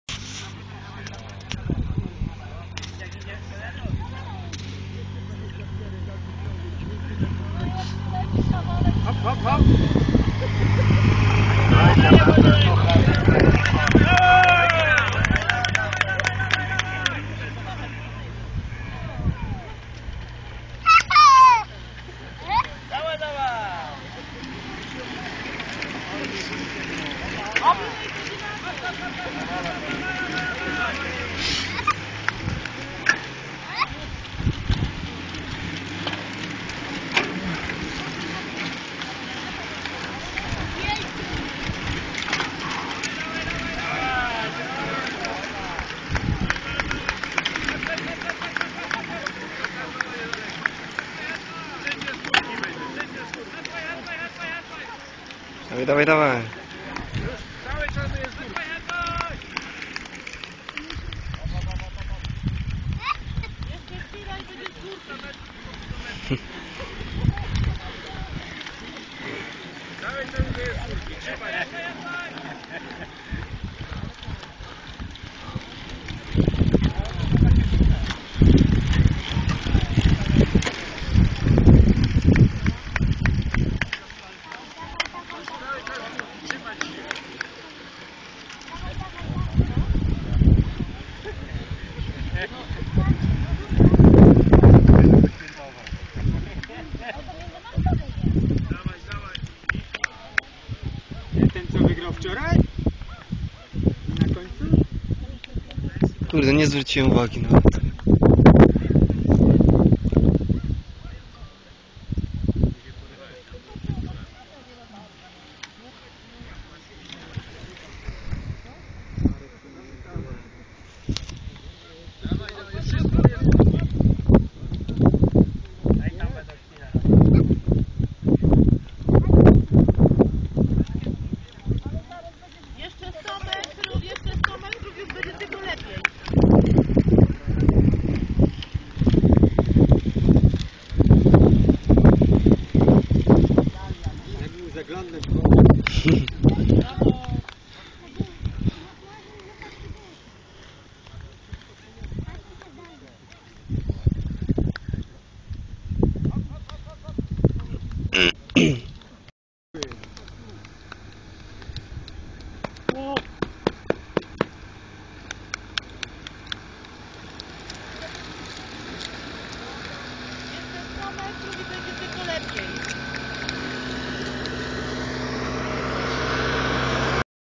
Karpacki Wyścig Kurierów - podjazd pod Kokocz 2014
Podjazd pod górę Kokocz od Woli Lubeckiej. Ostatni etap, 4.05.14r.
Słaba jakość..